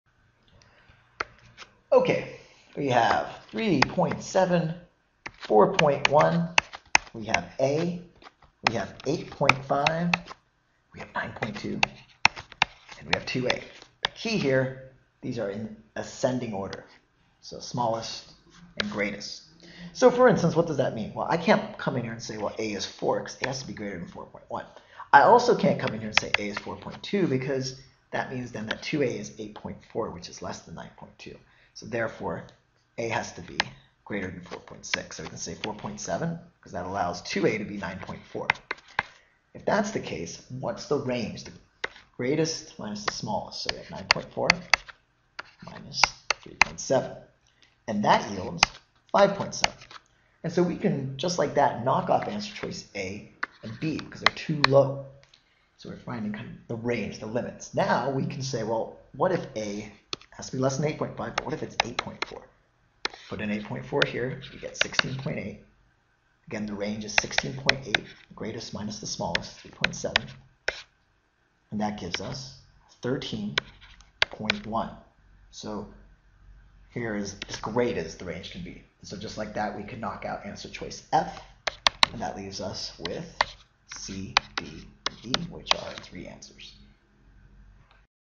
Revised GRE Practice Test 2nd Edition: Section 6 - Question 25
These are video explanations for the first edition of the ETS Practice Book for the Paper-Based GRE Revised General Test, 2nd Edition, which you can find

00:00 - Question 25
00:38 - Finding 2a
00:53 - Eliminating answers